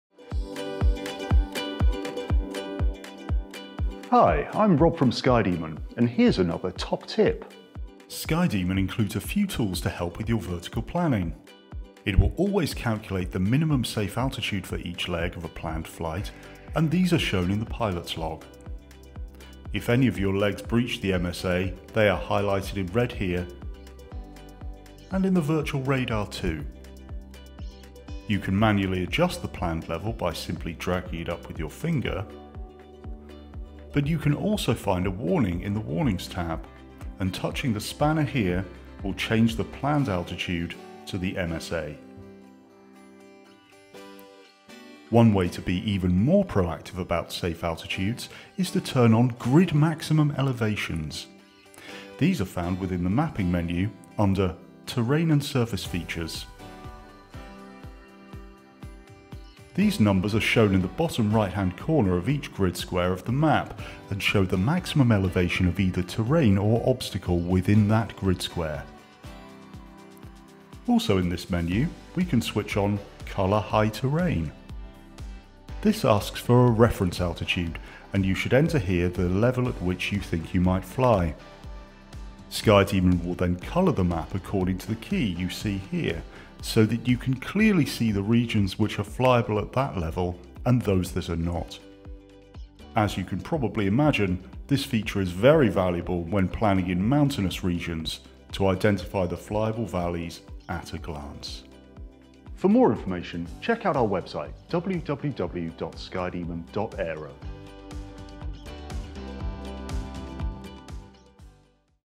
Safe Altitude Planning
This week Rob shows us three ways to keep your altitude safe when planning in SkyDemon: minimum safe altitudes calculated per leg, and adjusting each leg so it's safe; grid maximum elevation figures for a wider picture; and colouring high terrain for fine-grained planning in mountains and valleys.